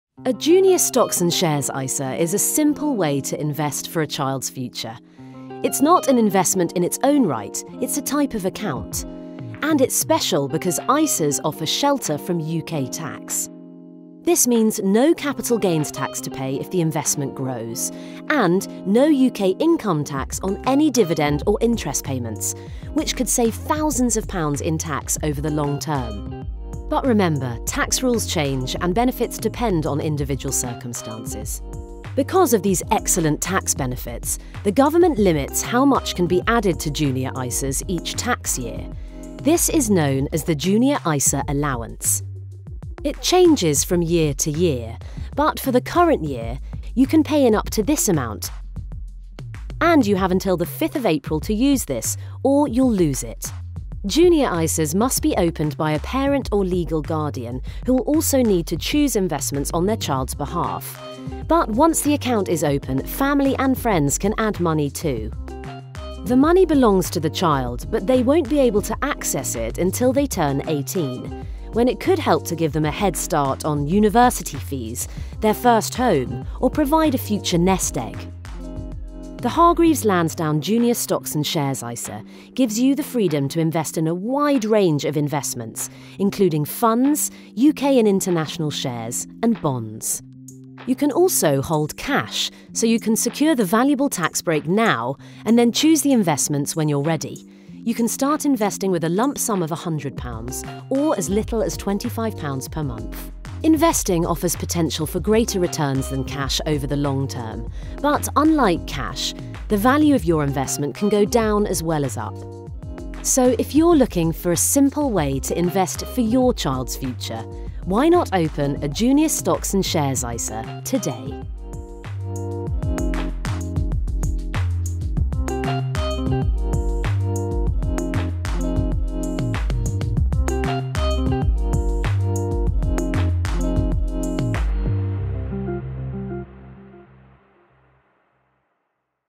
Junior Stocks & Shares ISA
ISAs are one of the most popular ways to save tax, and a great way to save for your child's future. Discover how Junior ISAs work and their key benefits.

00:00 Intro
00:05 Invest for a child's future
00:10 Tax benefits
00:30 Contribution limits
00:55 Who can open a JISA
01:07 Accessing a JISA
01:22 JISA Investments
01:38 How much you can start with

Tax rules can change and benefits will depend on circumstances.